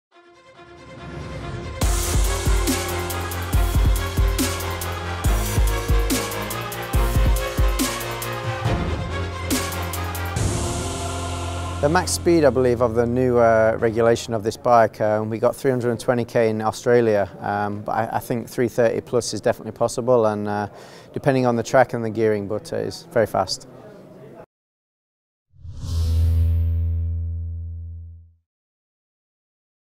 Leon Haslam Aprilia SBK 2015: the max speed in SBK
What is Aprilia max speed in SBK?
Rider Leon Haslam answers the question of our fan Arnaldo Plini.

Aprilia Racing Team SBK 2015 responds to fans’ questions collected on Facebook and Twitter

Learn more about our new adventure on the official website bearacer.com